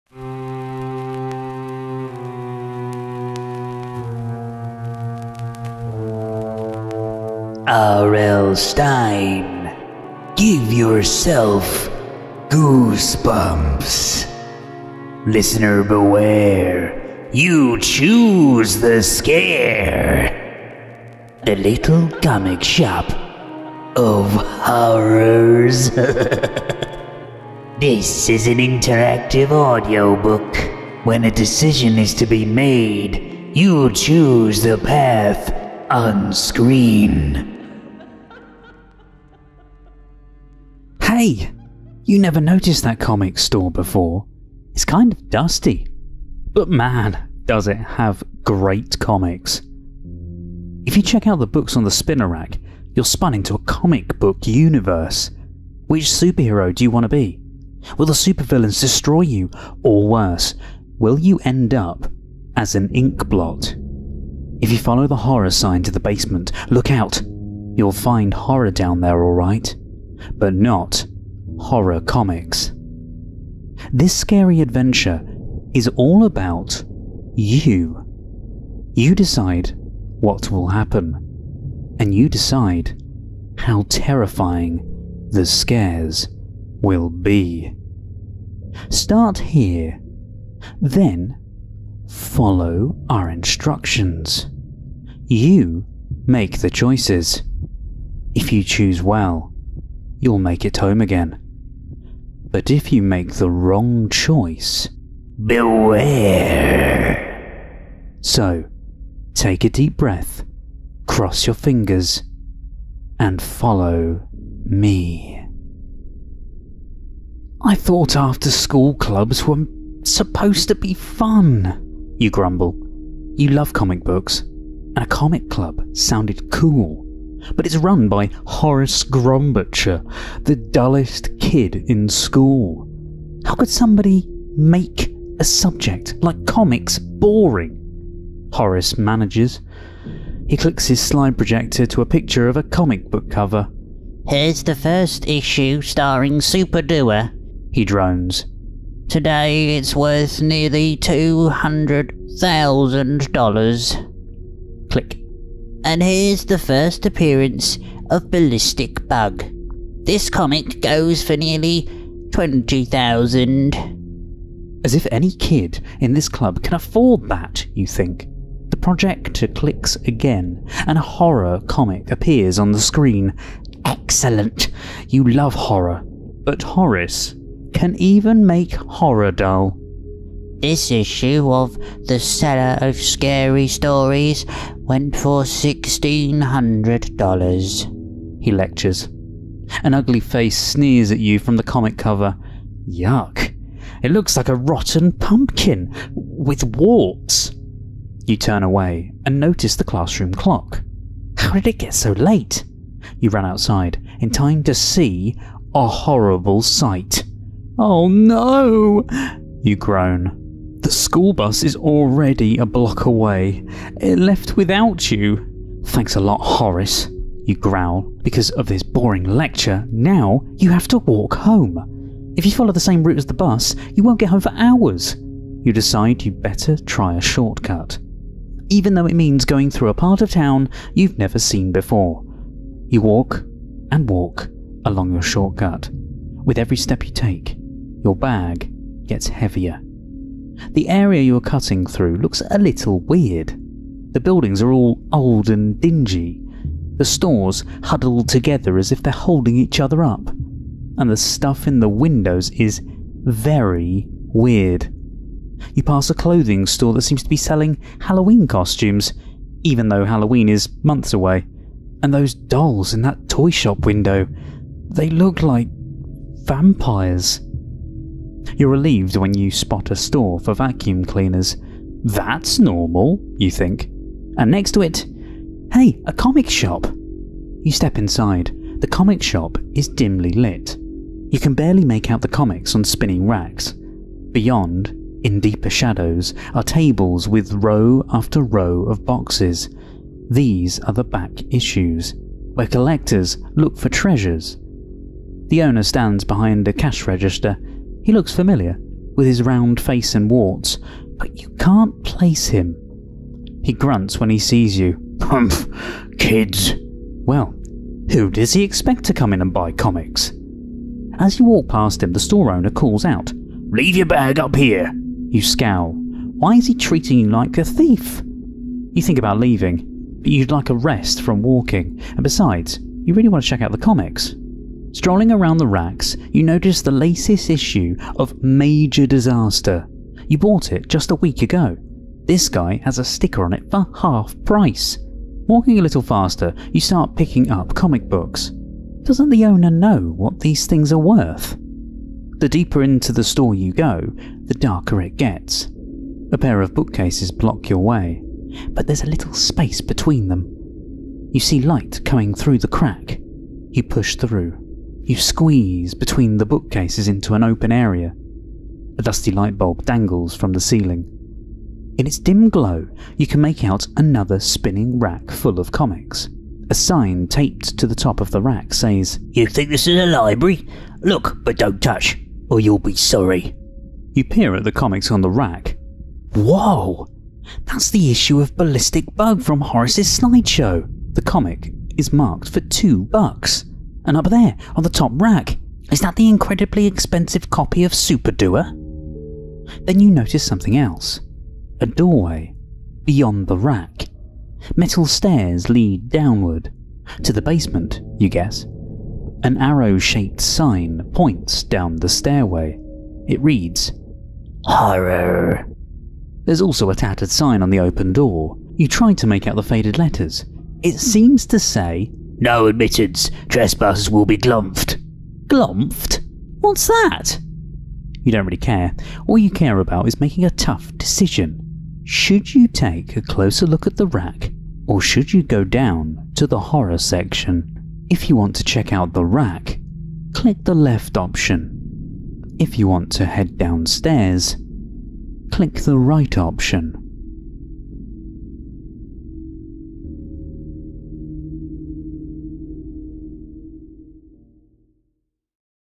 *MULTIPLE ENDINGS* Give Yourself GOOSEBUMPS: Little Comic Shop Of Horrors by R.L. Stine (Audiobook)
32 ENDINGS! Choose your own adventure and ending, with this INTERACTIVE "Give Yourself Goosebumps" audiobook. TO NOTE: Choices are at the end of the video, you choose your path.
This one consists of 66 different videos leading to 32 endings.
This took a long time to make, so sharing this with others that love Goosebumps would be really appreciated.
If you do notice broken links at the end of videos, please let me know!